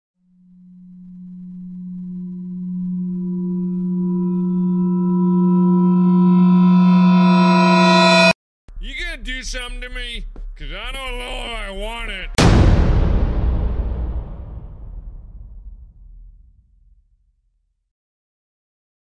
Its been a while.
Half way through my Something Else Production's animation, I start to clear space on my hardrive by deleting unwanted files. Including the files for the animation. "Well Heck." I said as I watched the months of work be obliterated from existence. However this gave me a fresh new start to actually learn editing techniques as well as ways to improve my work. I still don't have a decent camera though, oh well. note- I have not forgotten about something else productions.